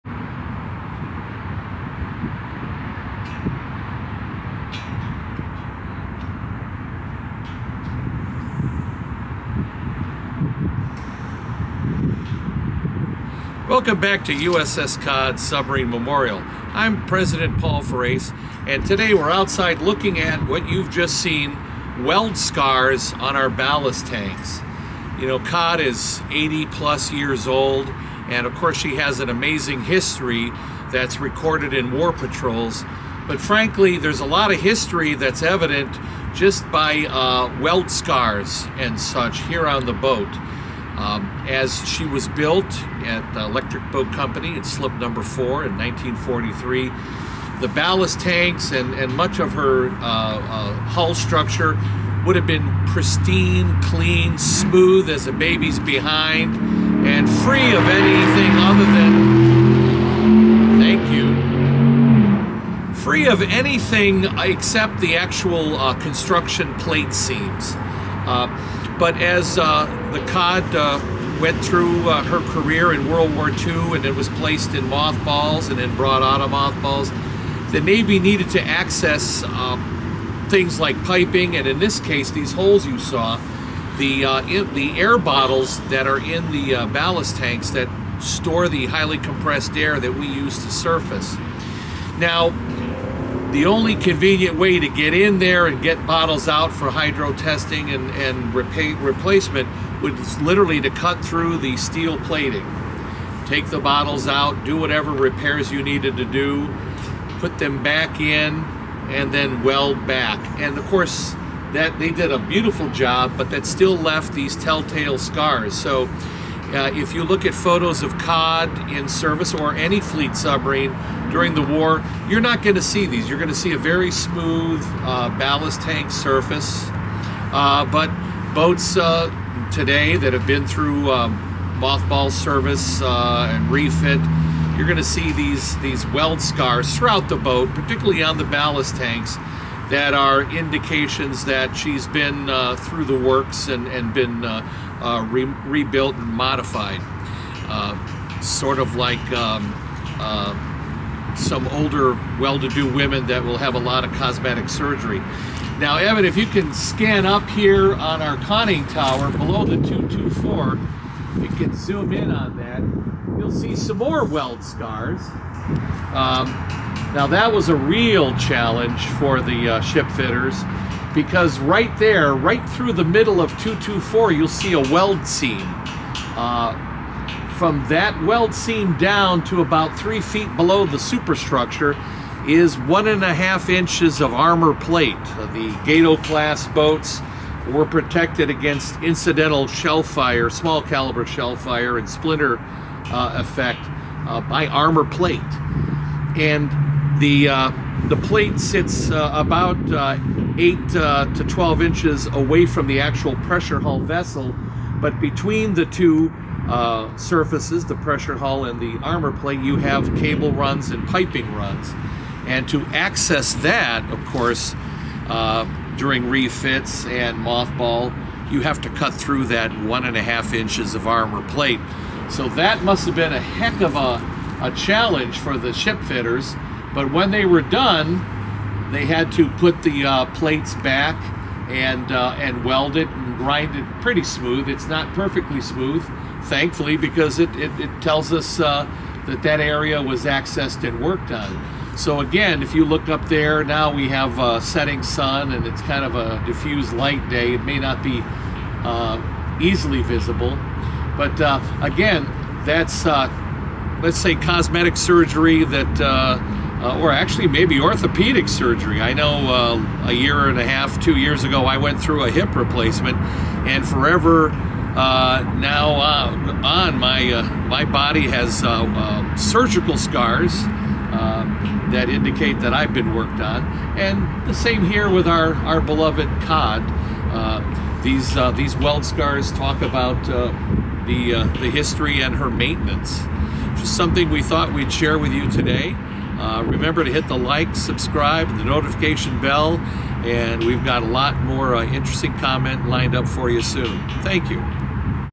Weld Scars Speak Volumes: How a Submarine’s History is Revealed by Weld Marks
WWII Fleet submarines like USS Cod were in and out of service for a number of years and underwent several overhauls and modifications. That work is often lost to contemporary stewards without access to detailed shipyard work orders. But the evidence of the work is to be found in the hull and superstructure. The newly built USS Cod was a sleek and smooth warship. Today, after years of service the multitude of welding scars on her ballast tanks and superstructure speak to work done to her in various Navy shipyards. Join Paul Farace as he looks at some of the evidence of Cod's service history recorded in her hull.